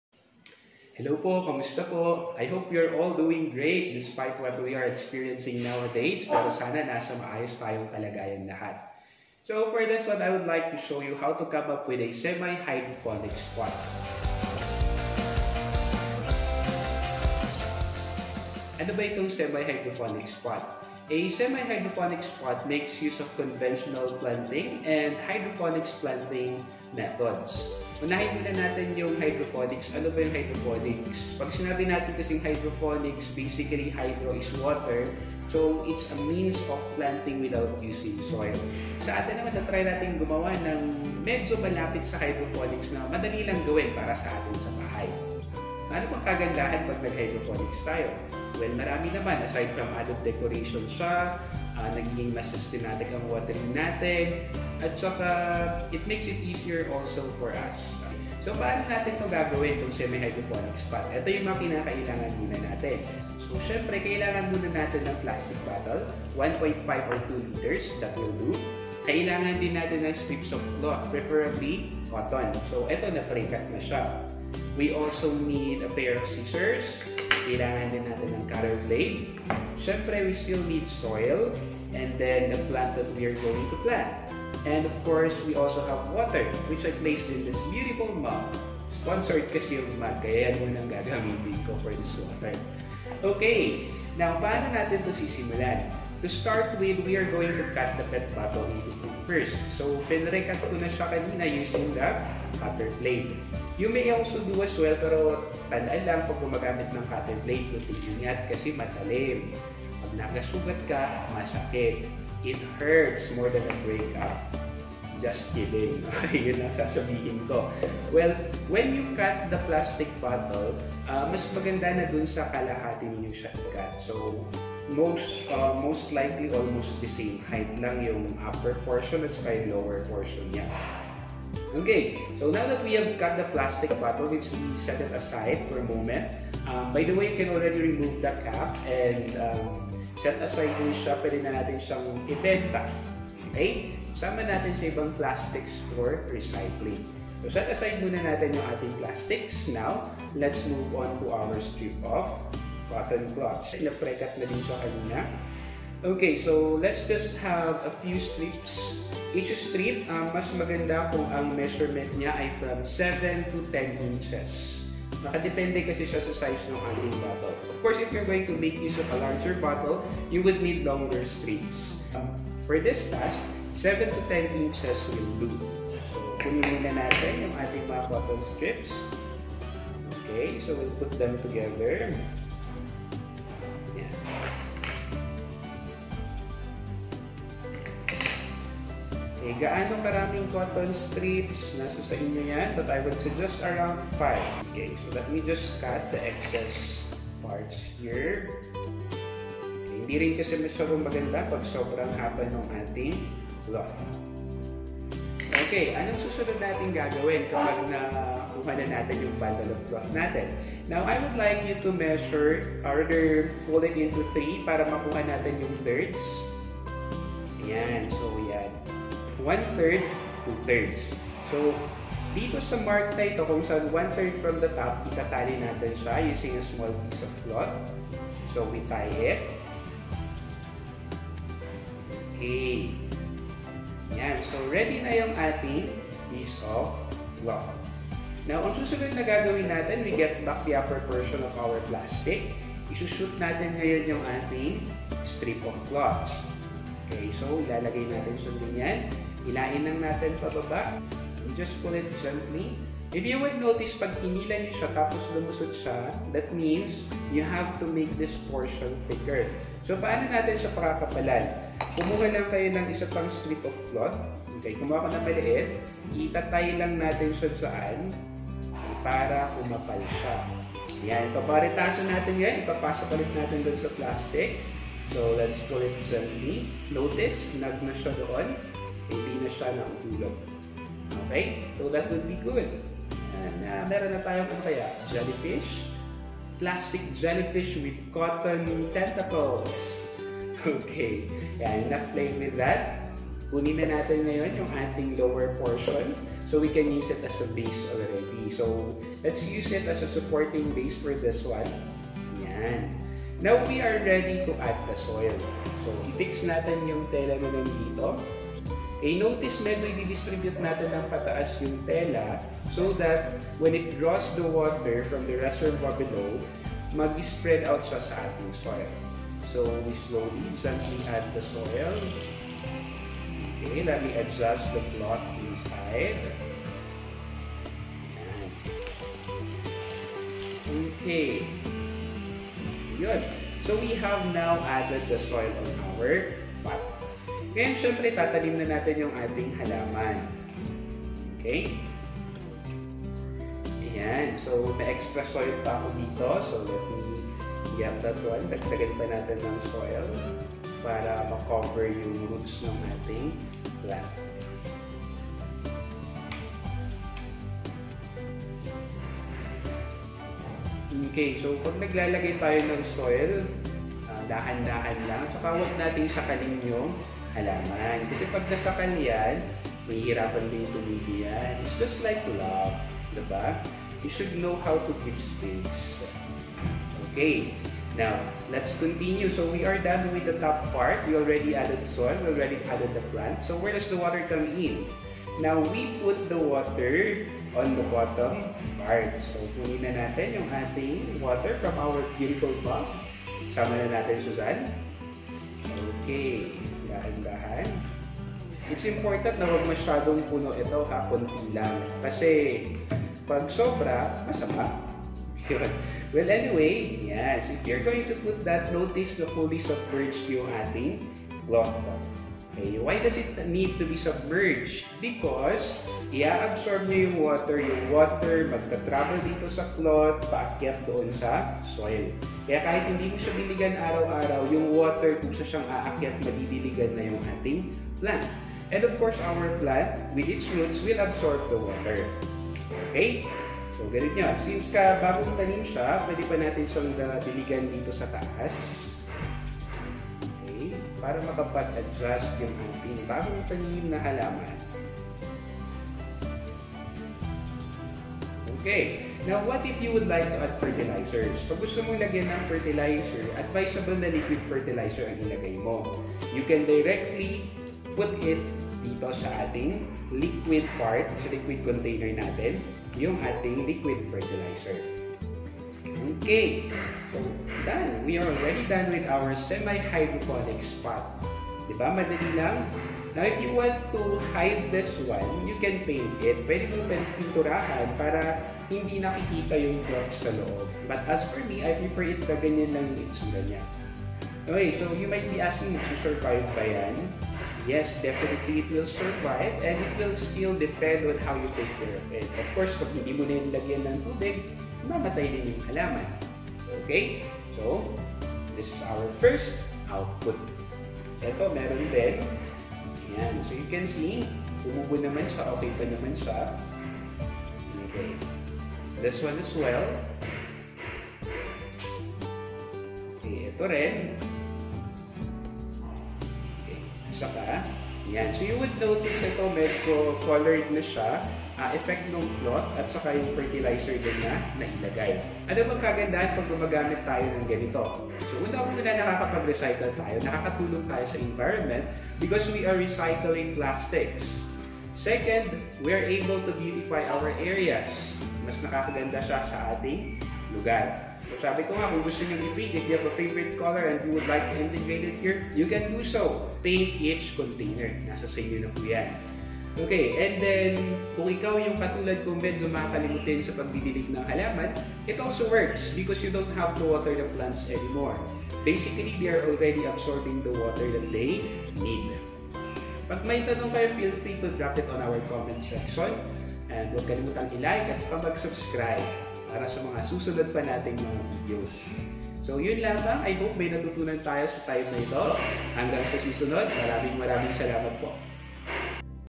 How to build a DIY Semi-hydroponics Pot
Hydroponics is a method of growing plants without usung soil.

In this video, we present an alternate means of planting that integratessome concepts of hydroponics. However, it's not a true hydroponic set-up since soil is still used, although the principle of capillary action is still taken advantage of. To make your set-up closer to full hydroponics, replace the soil with other materials such as pebbles, stones, marbles, or others.

Have fun!